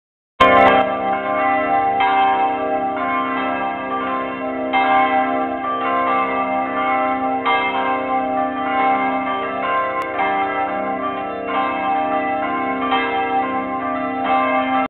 bells swinging
bell tower with swinging bells